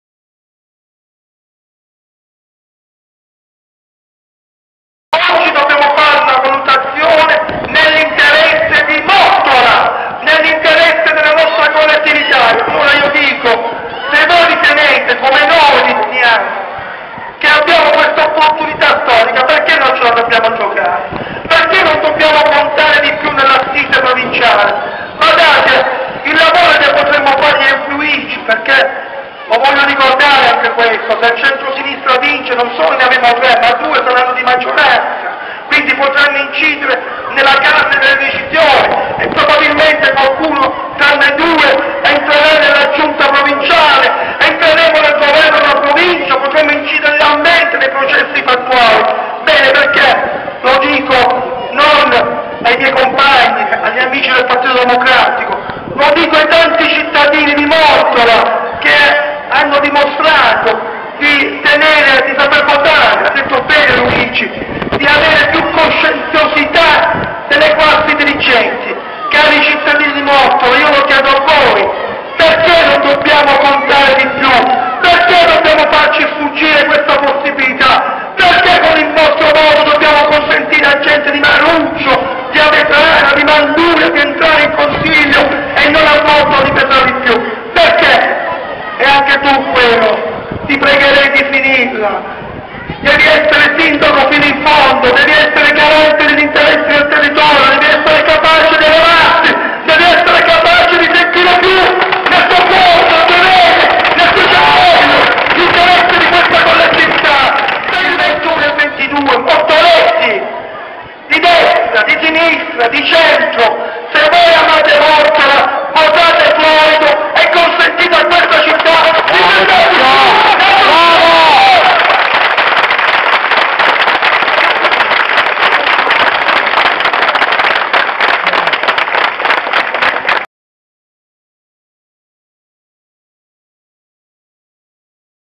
BALLOTTAGGIO APPELLO AL VOTO
Il 21 e 22 Giugno andiamo tutti a votare, scegliamo per il bene di Mottola, votiamo GIANNI FLORIDO

In questo modo Mottola avrebbe per la prima volta tre consiglieri provinciali di cui due di maggioranza.

Questo permetterebbe alla nostra città di pesare  di più nelle scelte della provincia nei prossimi 5 anni.

C'è bisogno di un segnale forte, usiamo il passaparola, sms, mail e soprattutto facebook.

Convinciamo, in queste ultime ore, quanta più gente possibile.